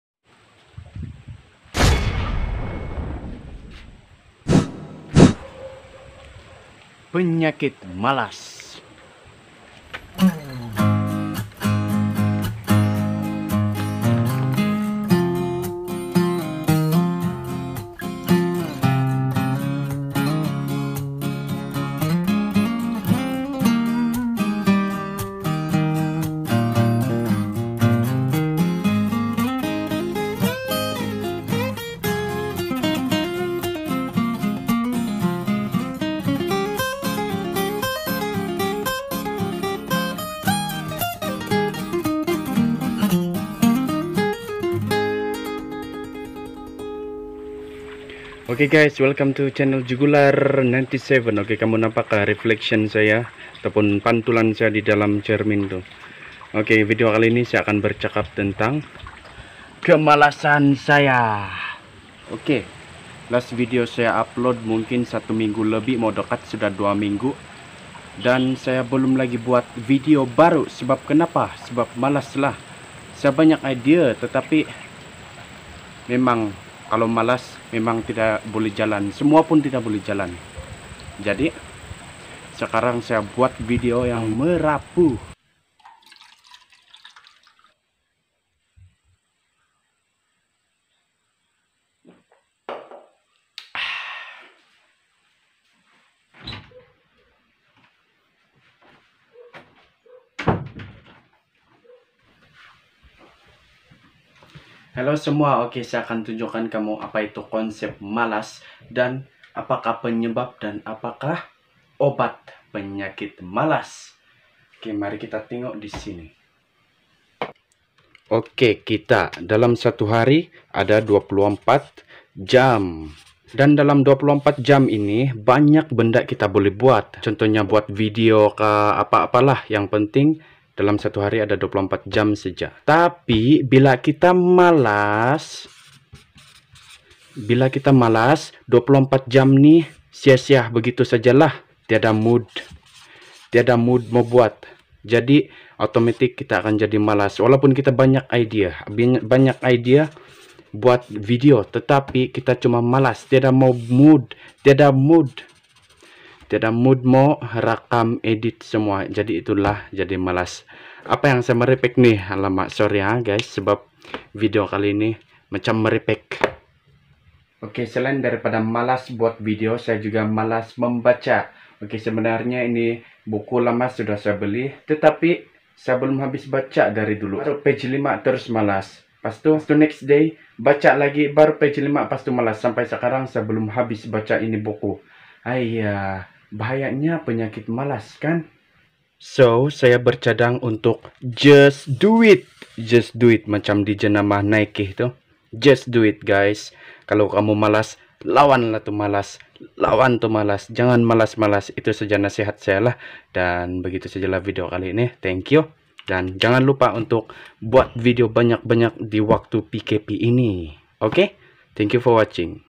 PENYAKIT MALAS!
Hi Geng!

Video pada kali ini adalah berkenaan kemalasan. Video ini ringkas dan merapu. Untuk menjadi konsisten dalam dunia youtube agak susah bagi saya tapi saya try seja untuk buat video meskipun malas. Sorry kalau membosankan haha. Pandai rindu edit video juga ni kadang².

Jugular 97